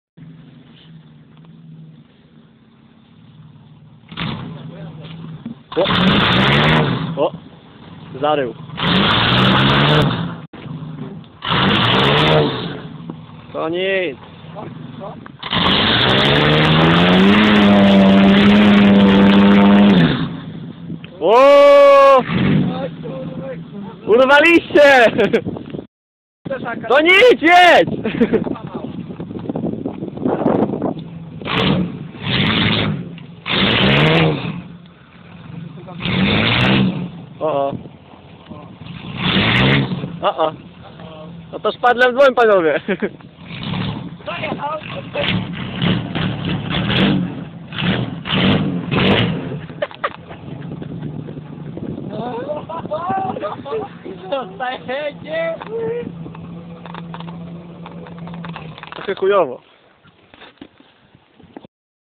Piaskownica część 2 Fordem Explorerem
No i ugrzązł :) Ale to niiic :P